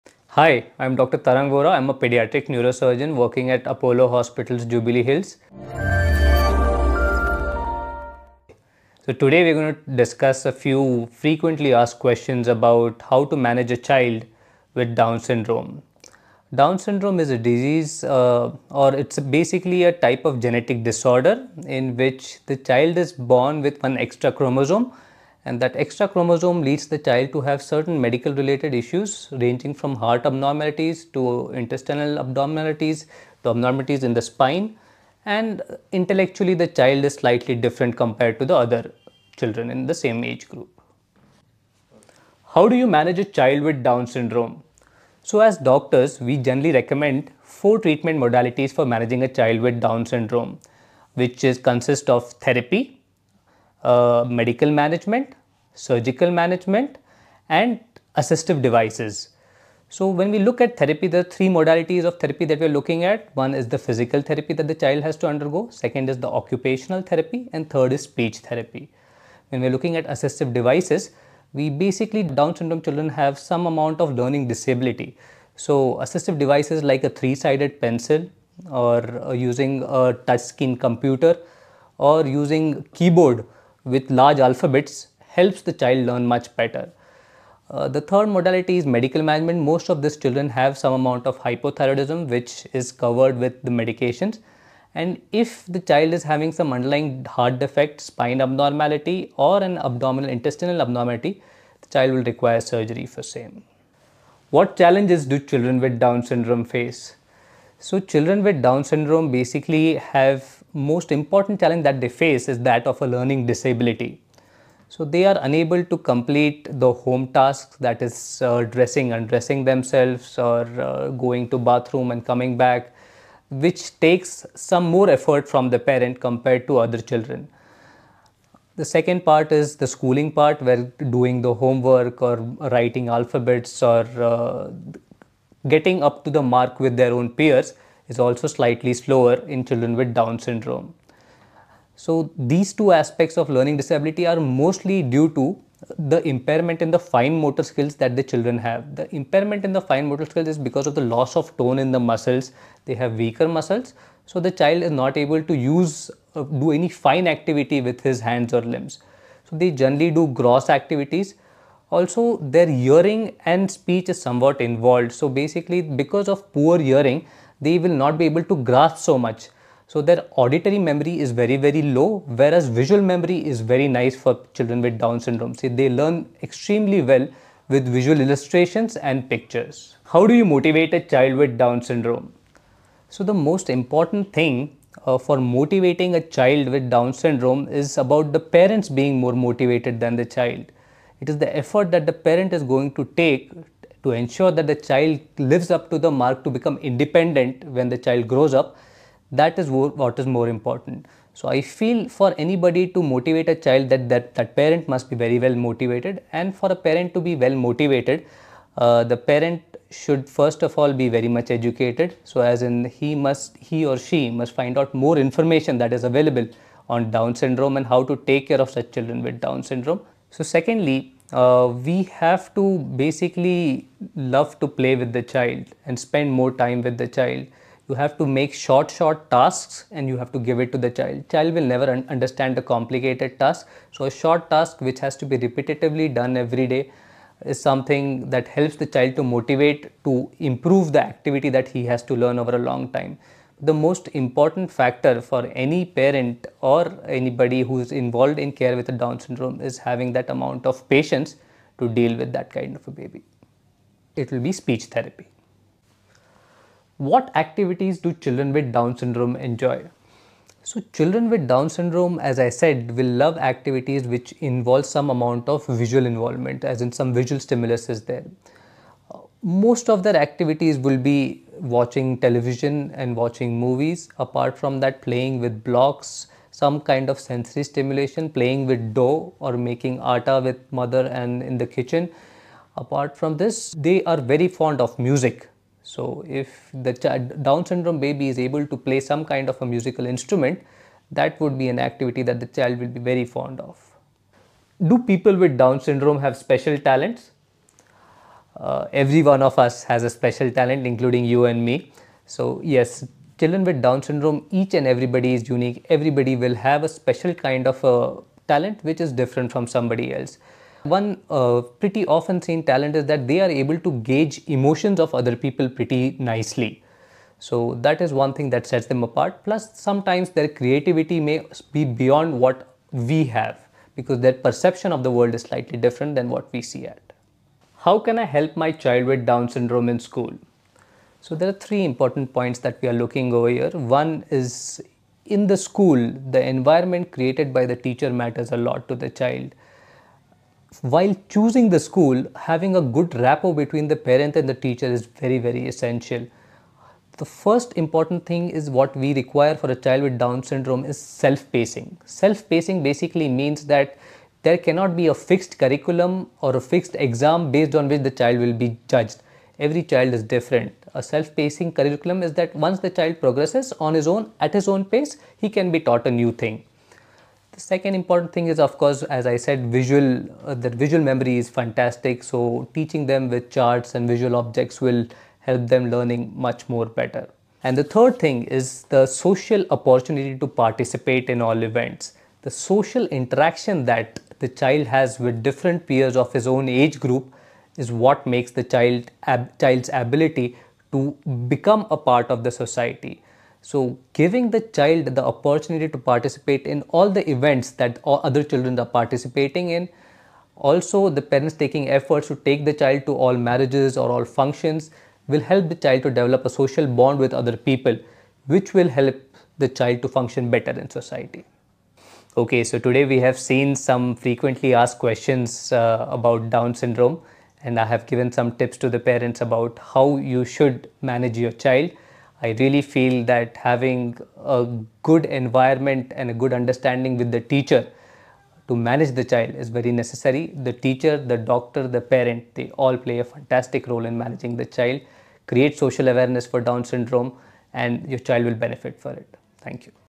Apollo Hospitals | Down Syndrome | Dr. Tarang Vora
Our latest video on "Down Syndrome" is by renowned Dr. Tarang Vora, Pediatric Neurosurgeon at Apollo Hospitals, Jubilee Hills, Hyderabad, and experienced in the respective field.

The expert answers the commonly searched questions surrounding Down Syndrome, a genetic disorder leading to developmental problems.

0.00 - Introduction
0.16 - About Down Syndrome
0.41 - Management of child with Down Syndrome
1.43 - Challenges to face child with Down Syndrome
3.00 - Motivation for child with Down Syndrome
4.16 - Activities for child with Down Syndrome
4.54 - Special talents in child with Down Syndrome
5.27 - Help for child with Down Syndrome in school
6.52 - Conclusion